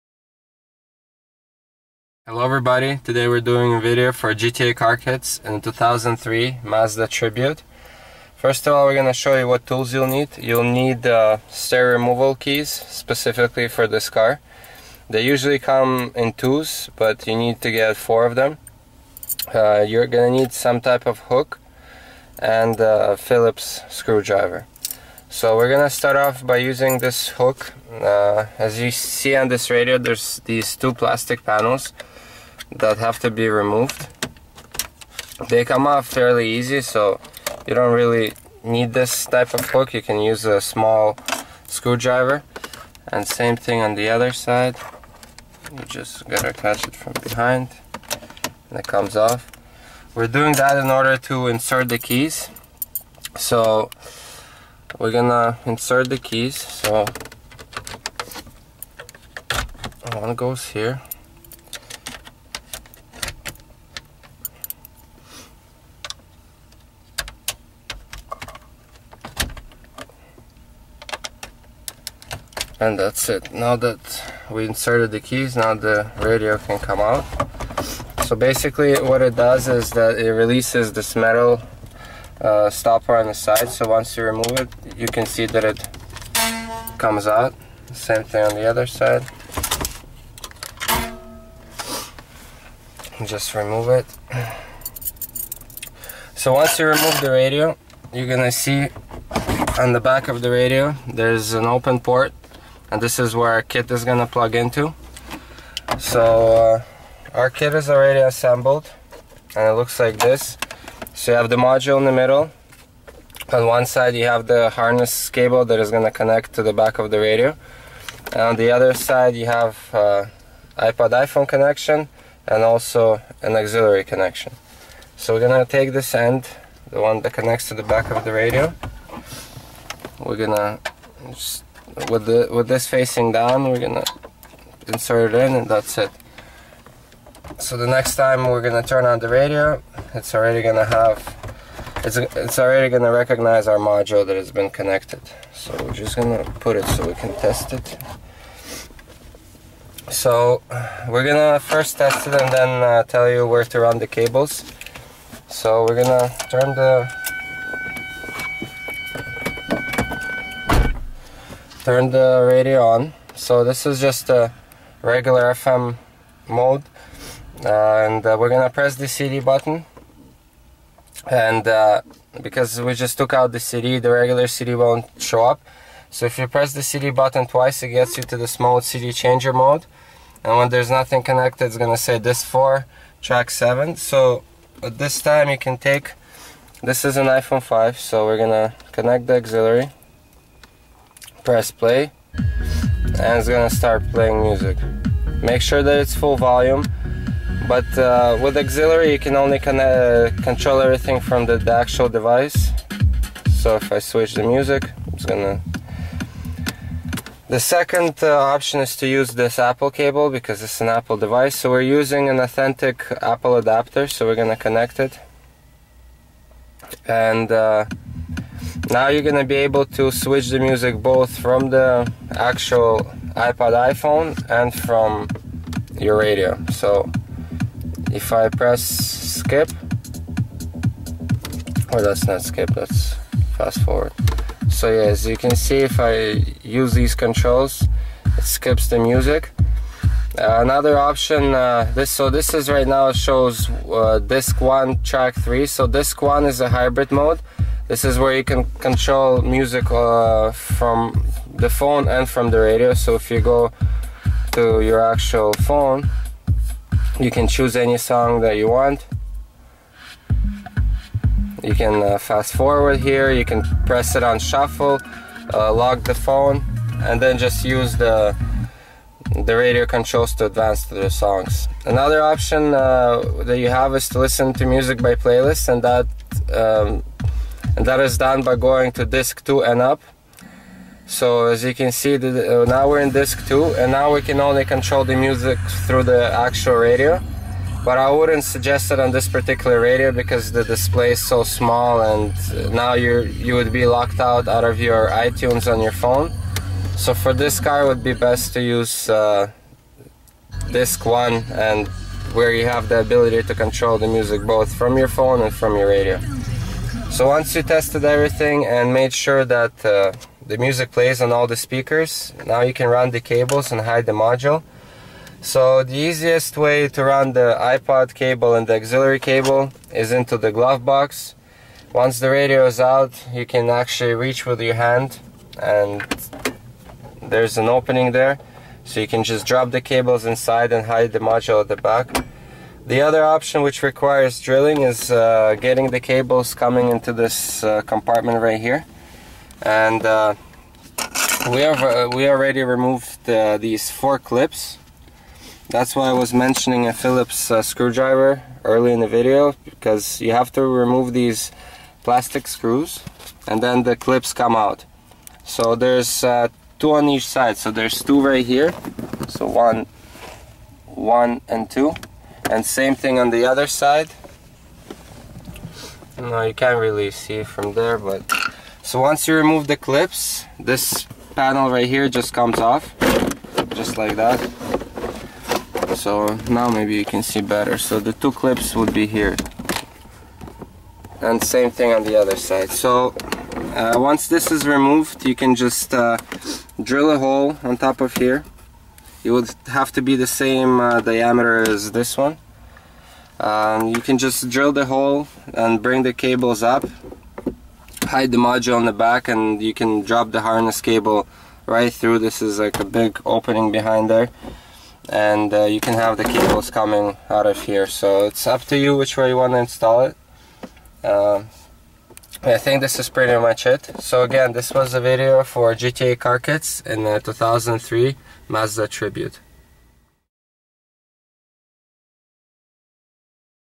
GTA Car Kits - Mazda Tribute 2002-2006 iPod, iPhone and AUX adapter installation
- installation instructions for iPhone, iPod, iPad, AUX, mp3 adapter into factory stereo of Mazda Tribute for model years 2002, 2003, 2004, 2005 and 2006. After installing the car kit you would be able to listen to your iPod or iPhone through your factory stereo speakers and control your music through controls on the factory radio along with displaying the song name. The kit will charge your iPod/iPhone.
You can use these instruction for removing Mazda Tribute stereo unit.